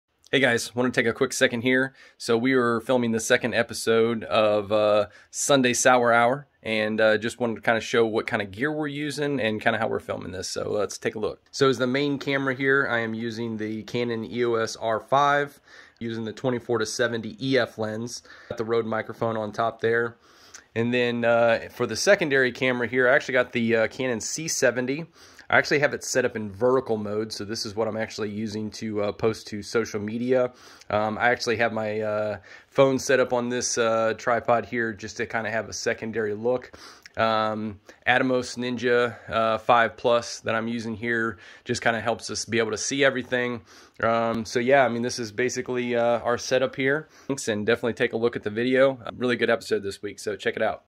Quick look at the gear used for the new Sunday Sour Hour episode live now!
New Sunday Sour Hour episode live on the YouTube channel now! Here's a look at some of the BTS gear used to film this episode.